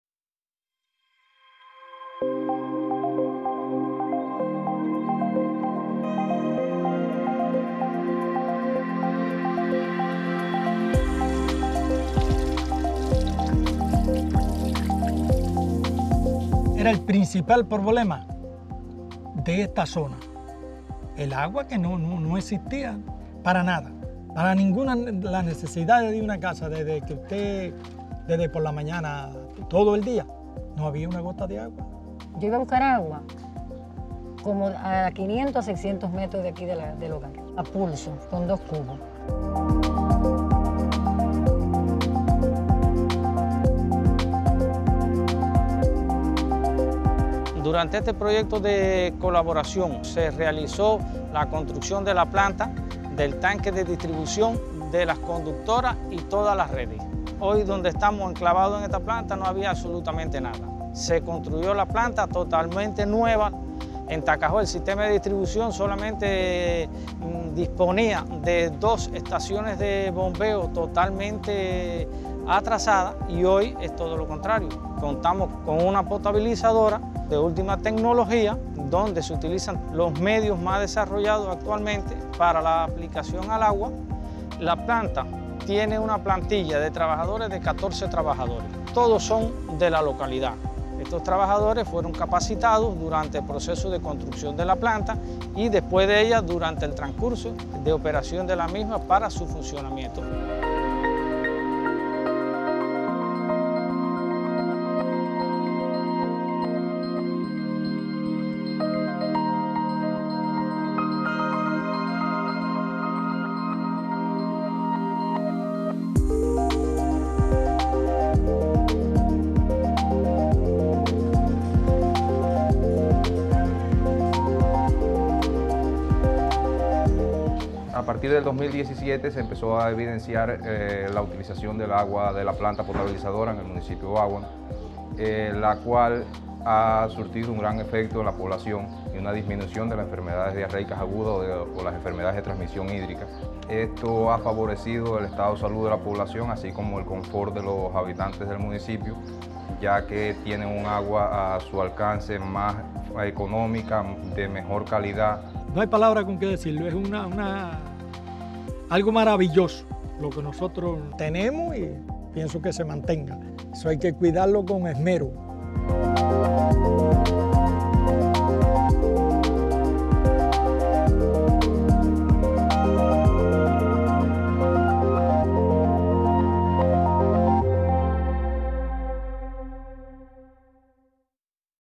Proyecto de agua y saneamiento en Báguanos
El agua llega por fin a los hogares de Báguanos y Tacajó tras años de difícil acceso al vital líquido. Conoce cómo se ha desarrollado este proyecto del Fondo de Cooperación para Agua y Saneamiento en Cuba.